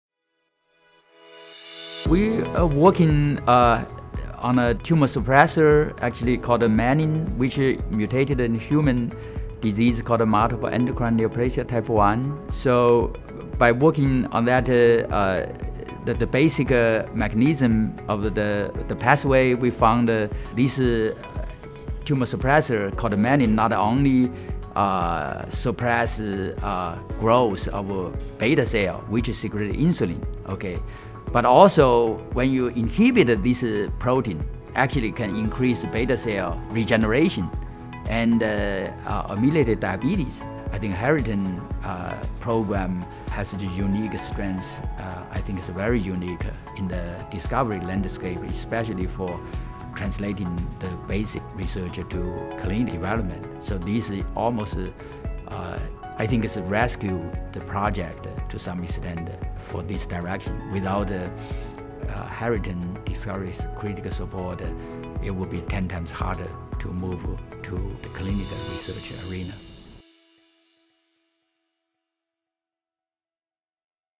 Pursuing a New Treatment for Diabetes
Dr. Hua is working on a tumor suppressor called Menin and its role in treating diabetes. Dr. Hua received a 2015 Harrington Scholar-Innovator Award to support his drug discovery and development.

Dr. Hua works at the University of Pennsylvania. The project selected for the Harrington Scholar-Innovator Award is “A drug that regenerates pancreas cells to treat diabetes.”